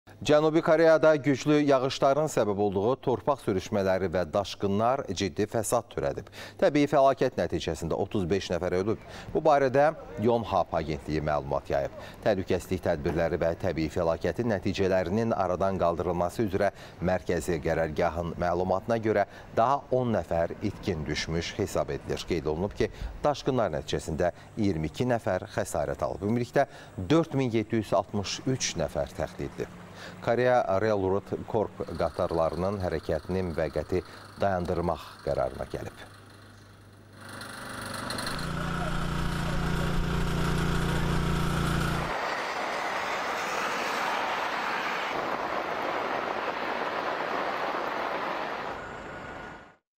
Güclü yağışlar dəhşətli fəsadlara səbəb oldu: təbii fəlakət nəticəsində 35 nəfər öldü
FƏLAKƏT GƏTİRƏN YAĞIŞLAR